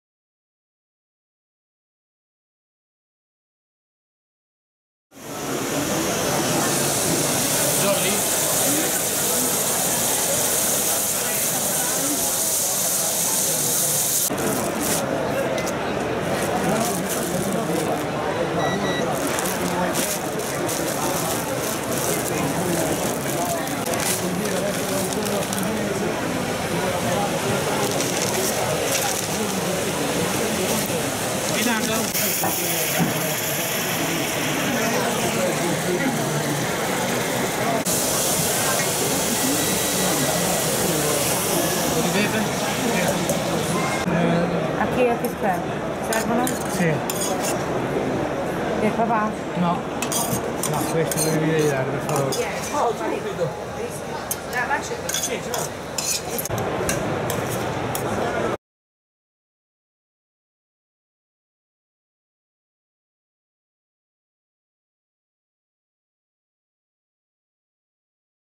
Come si cucina la Pasta di Campofilone
E’ una delle tipologie della pastificazione più richieste del momento, grazie al suo gusto caratteristico e alla versatilità in cucina: siamo andati a scoprire perché piace così tanto…